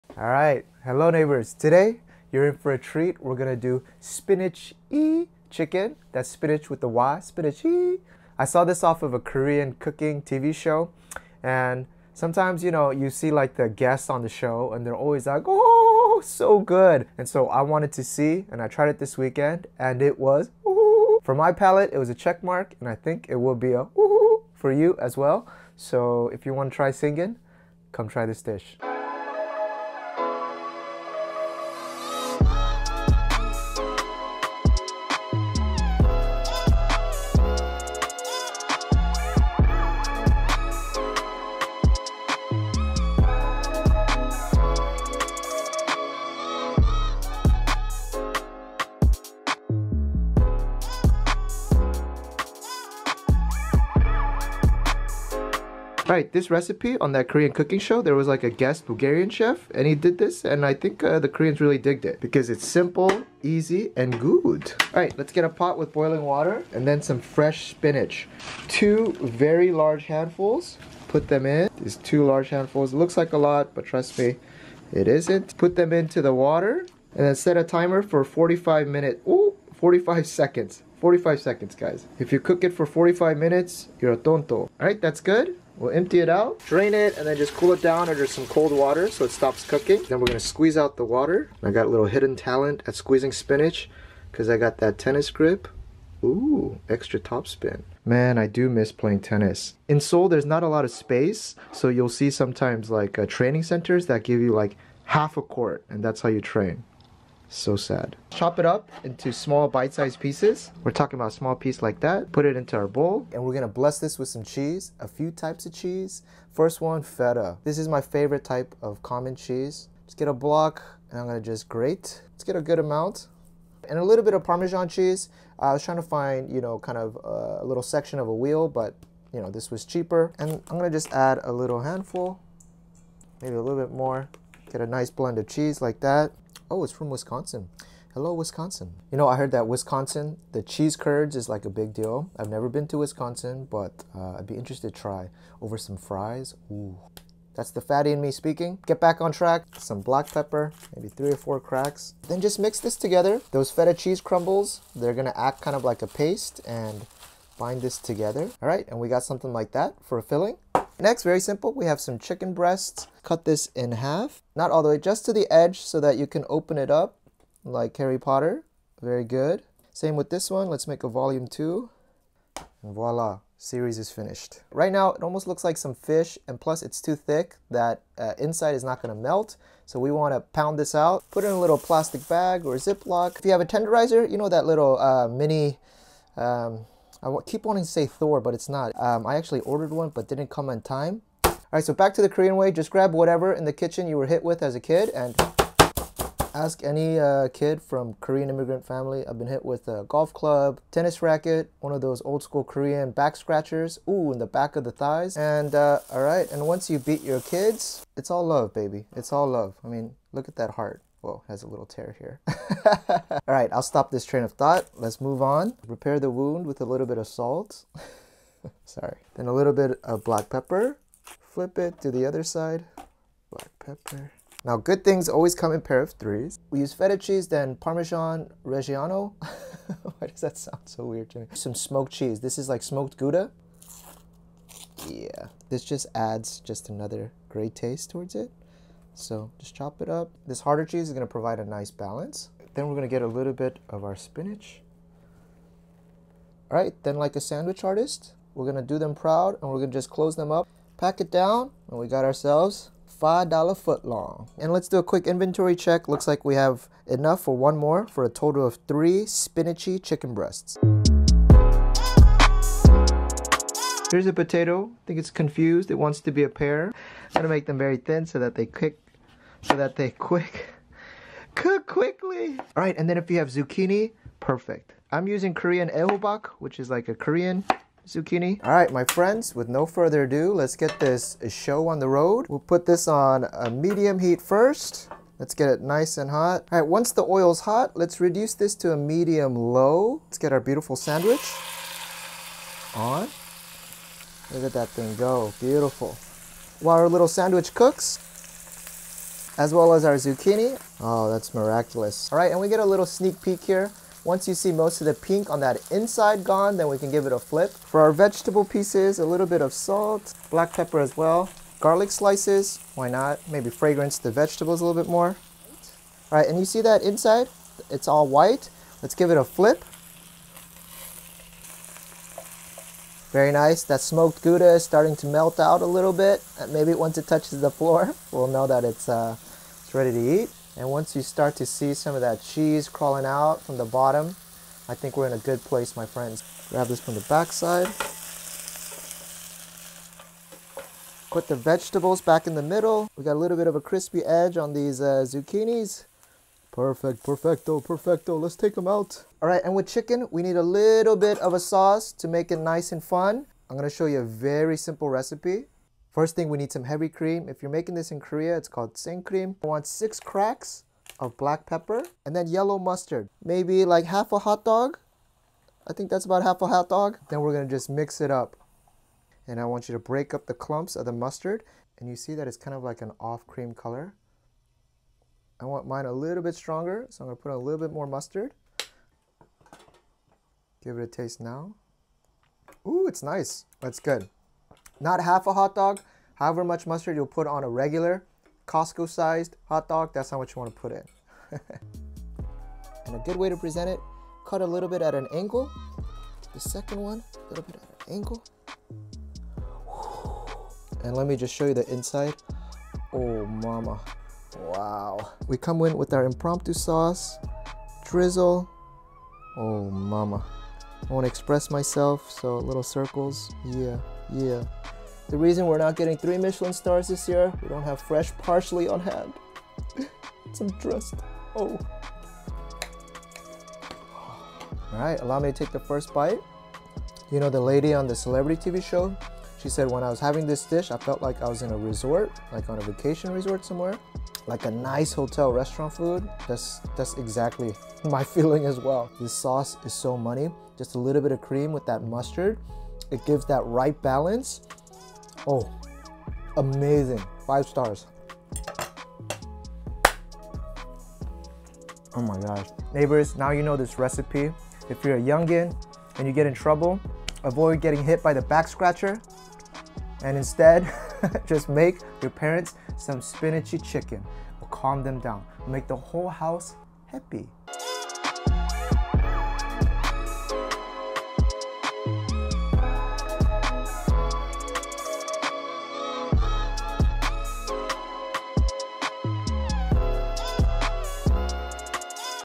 How to: Spinach & Smoked Gouda Chicken Breast
Today, we replicate a fun recipe that was shown on a Korean cooking channel called Sumin's Banchan (수미네 반차). It look'd so good on TV, that we tested it out over the weekend. The TV host wasn't lying when she said it tastes likes a meal from a luxury hotel! Heh... here it is!